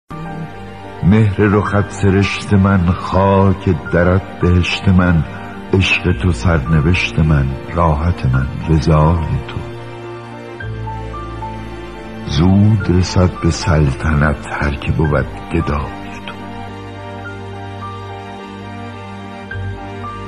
مهر رُخت سرشت من، خاک درت بهشت من، عشق تو سرنوشت من - حضرت آقای حاج دکتر نورعلی تابنده مجذوبعلیشاه ❤
هو
۱۲۱ ؛
مهر رُخت سرشت من،
خاک درت بهشت من،
عشق تو سرنوشت من،
راحت من رضای تو

حضرت آقای حاج دکتر نورعلی تابنده مجذوبعلیشاه اعلی الله مقامه الشریف❤